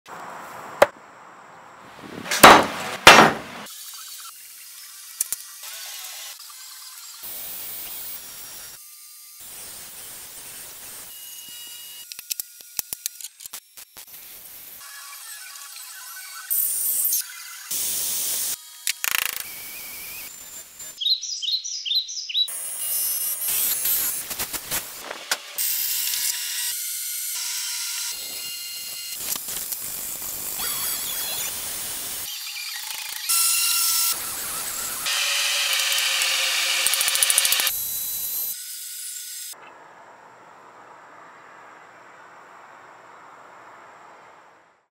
How to turn a $10 flea market find into… a cold rolled steel Hatchet!
@jimmydiresta I picked up this Hatchet Head for 10$... then made this handle out of cold rolled steel for the handle, to finish off this hatchet build!